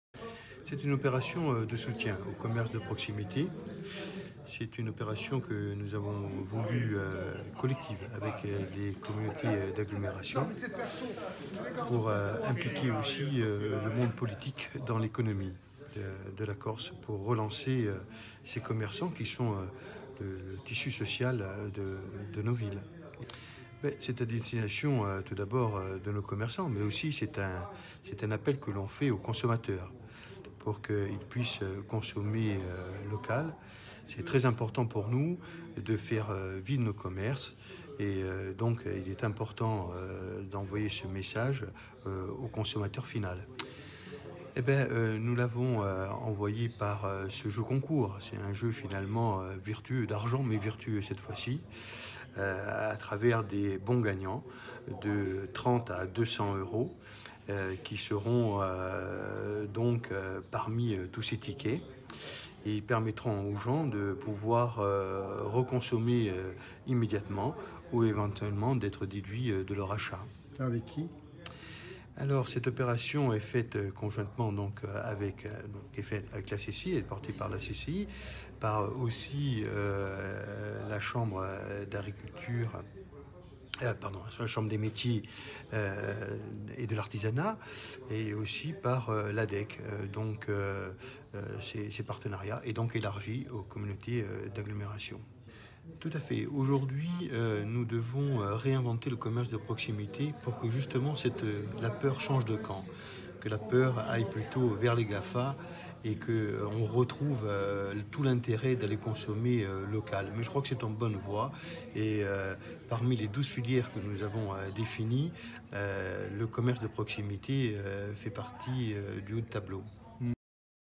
Corse : 293 000 € de bons d'achats à gagner chez les commerçants de proximité
L'opération "Compru Quì"organisée par la chambre de commerce de Corse avec l'agence de développement économique de la Corse et la chambre de métiers et de l'artisanat permettra aux consommateurs corses de gagner 293 000 € de bons d'achats - 9 000 bons d'achats de 30, 50, 100 et 200 € - auprès des commerçants et artisans de proximité de l'île. "Compru Quì",est également portée par plusieurs communautés de communes de l'île qui ont rejoint le mouvement. L'opération  démarre le 12 août et se poursuivra jusqu'au 27 septembre,